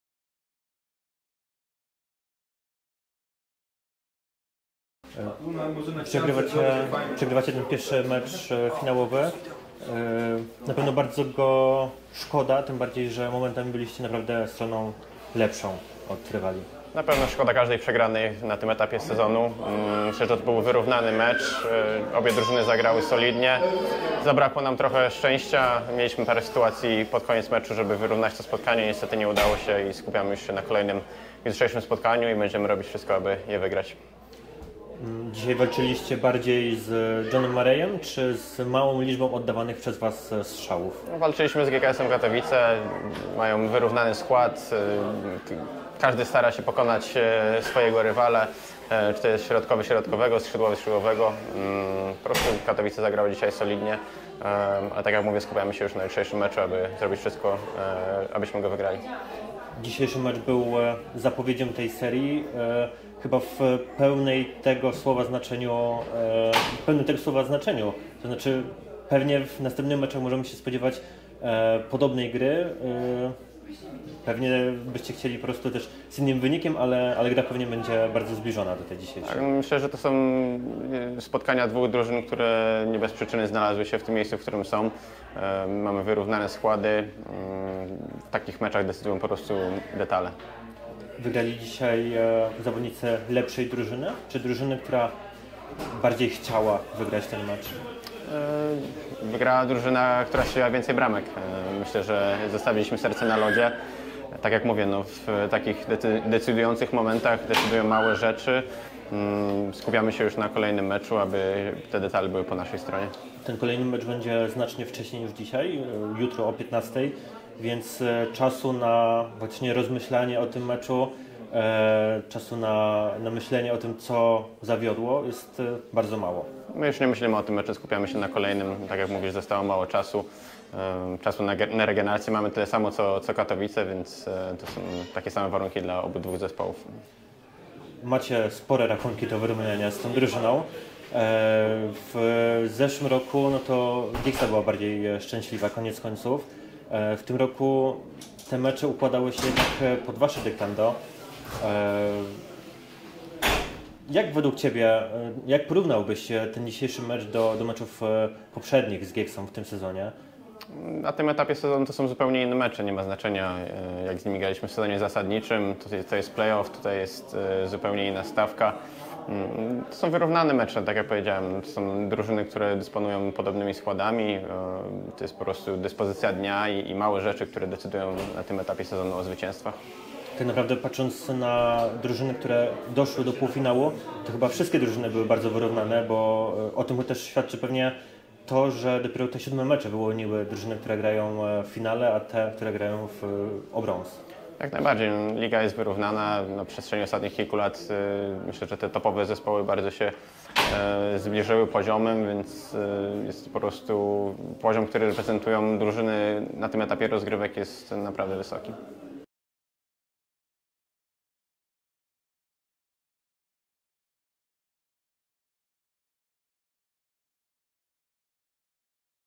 Filip Starzyński po meczu GKS Tychy - GKS Katowice
Nie udało się zawodnikom GKS-u Tychy wygrać pierwszego meczu finałowego Polskiej Hokej Ligi. Przed własną publicznością tyszanie przegrali to spotkanie 1:3. Po meczu z GKS-em Katowice zapytaliśmy Filipa Starzyńskiego m.in. o powody porażki, małą liczbę strzałów, a także o porównanie dotychczasowych spotkań z GieKSą.